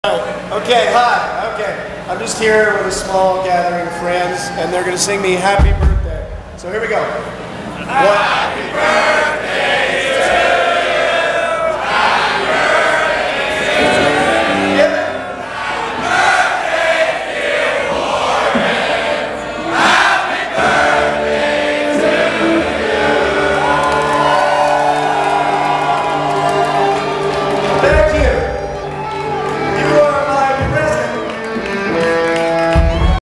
Warren Gets Birthday Wishes From Assembled Grit Youth
Yea, verily.  The annual Festival of Joy nears its end, culminating in the celebration of the birth of the Warren child.

Last night at the Rajin' Caygeon in Guelph, three hundred Grit youngsters gave voice to their joy.

It was a beautiful moment.